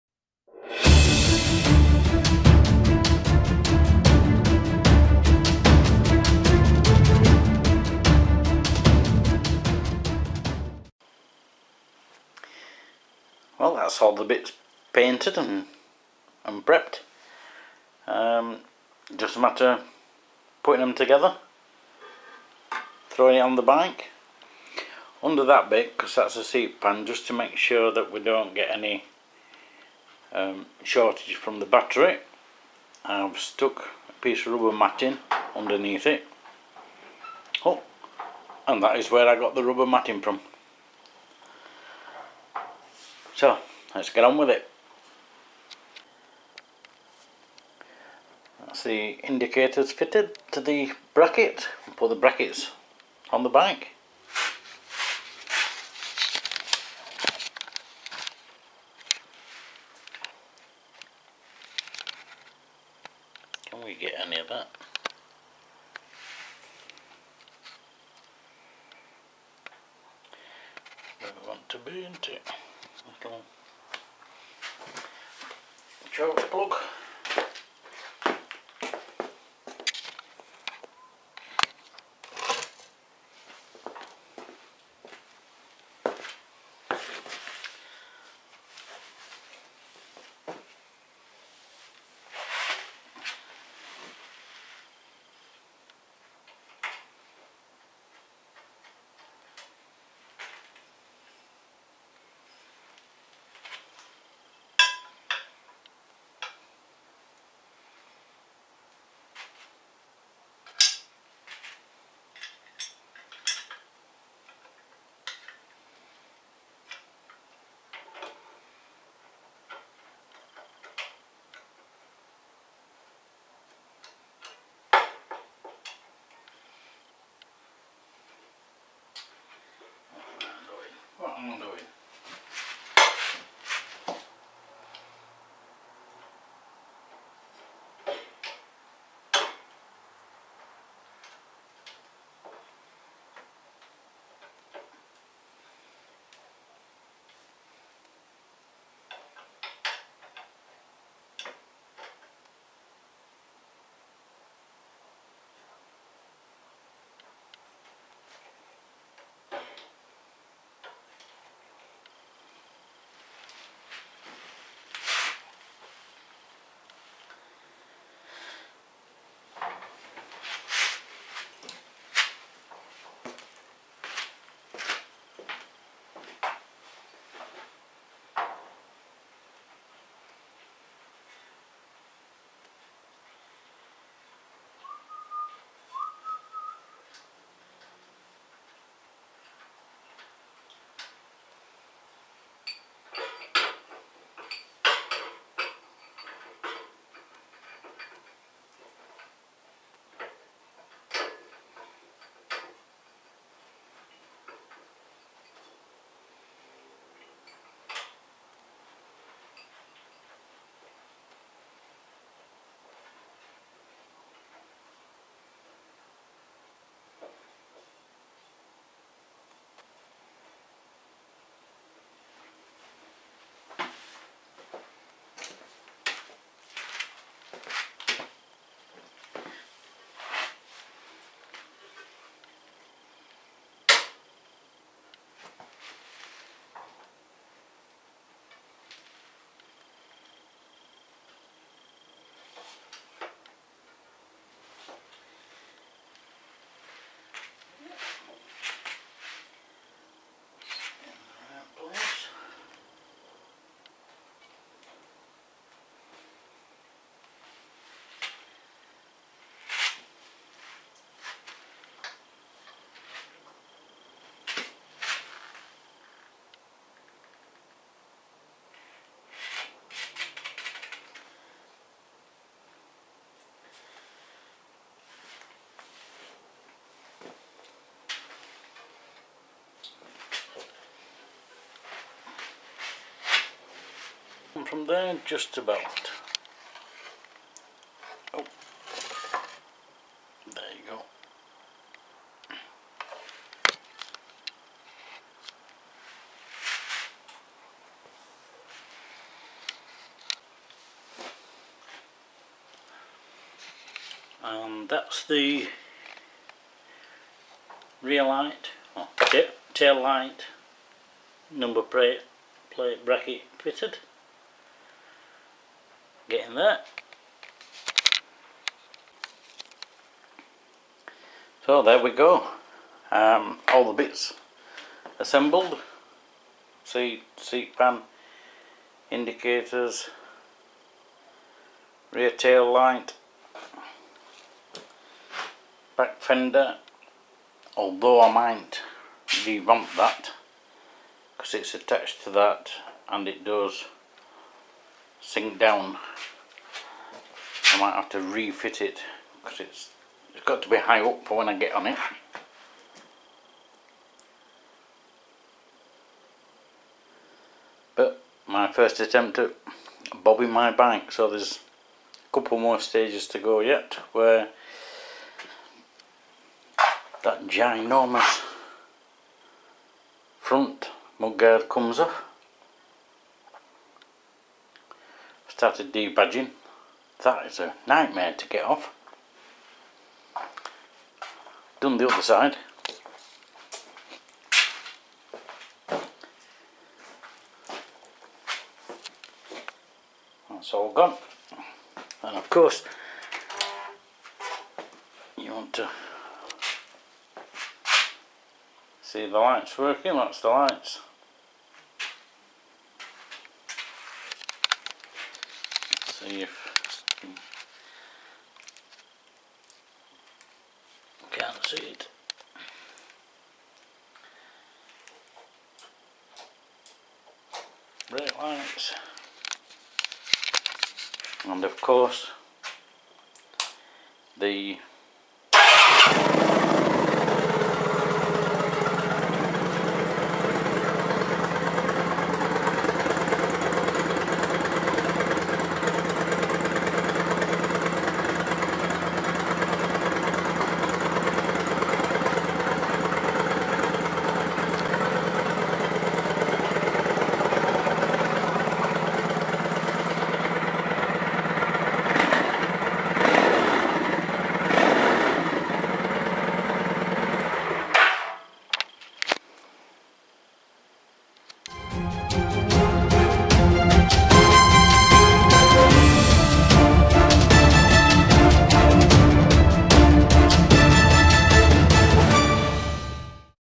Kawasaki VN900 to Bobber #5
Fitting all the bits onto the bike